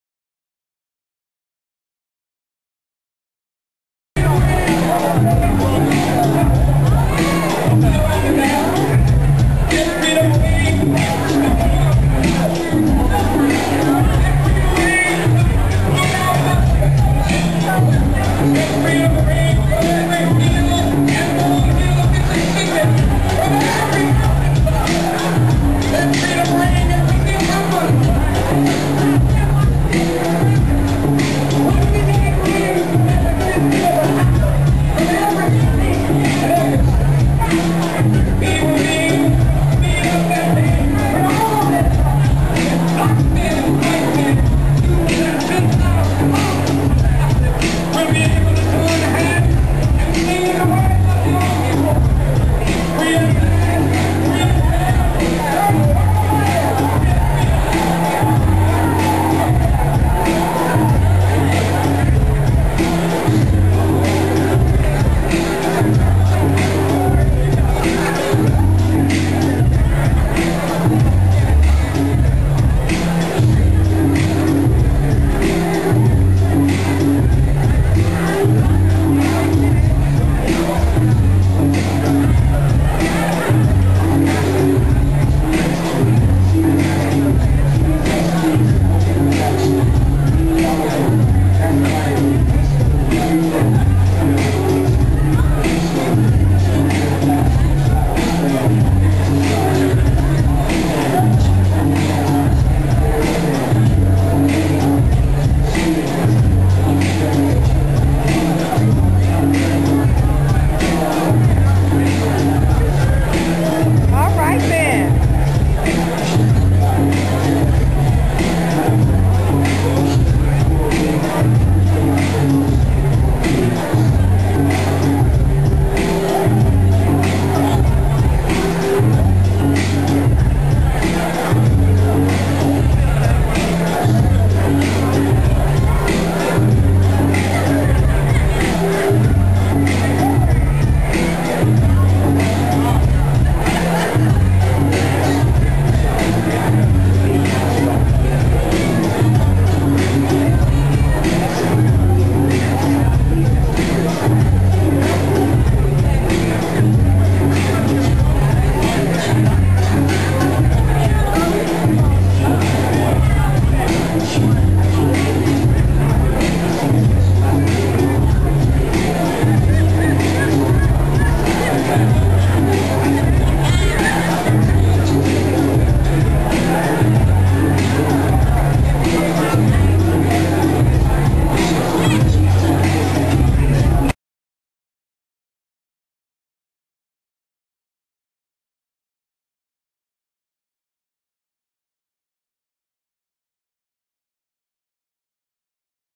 High Steppers Dance Troupe featuring Zeus, Houston
High Steppers dance instructor Henry Harvey a/k/a "Zeus" with Janice, a High Stepper. Zeus specializes in Houston 2-step, but here he and Janice are swinging, Dallas style.